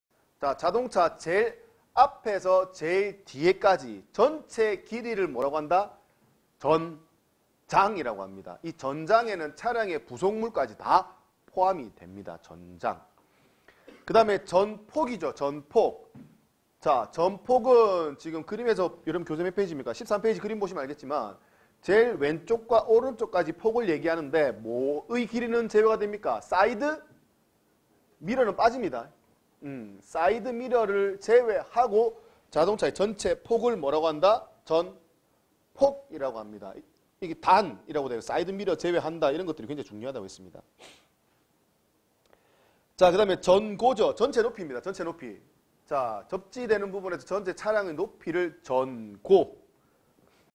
9급 운전직공무원 대비 '전장 및 전폭'
9급 운전직공무원 대비 단기속성 이론집중특강!
이윤승선생님의 자동차구조원리

최신개정법령완벽반영
3년 연속 도로교통법규 적중률100%
자동차구조원리 적중률 90%
최고의 합격 솔루션 제공!

2018, 2019 운전직공무원 준비는 와우고시에서!